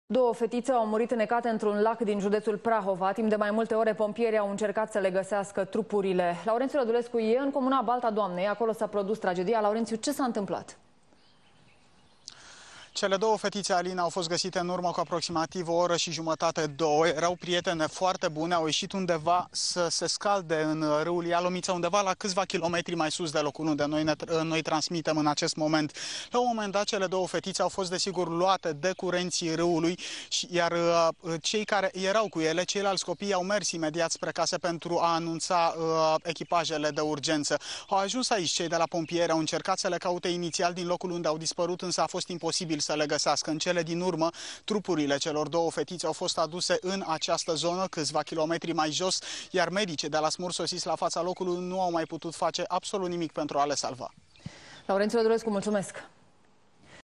Doua fetite au murit inecate in Prahova 10 IUNIE 2010
Doua fetite au murit inecate intr-un lac din judetul Prahova. Timp de mai multe ore pompierii au incercat sa gaseasca le gaseasca trupurile.